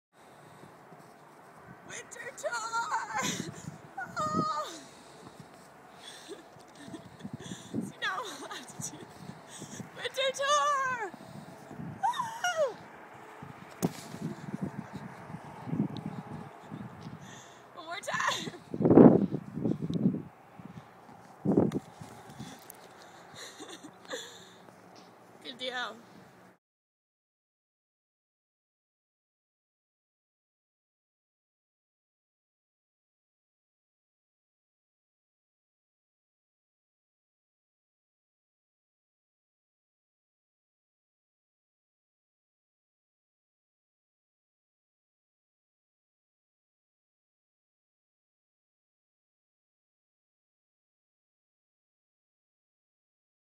Winter Tour - Snow
Having fun at a rest stop in the smoky mountains. I built a snow dog!!! :)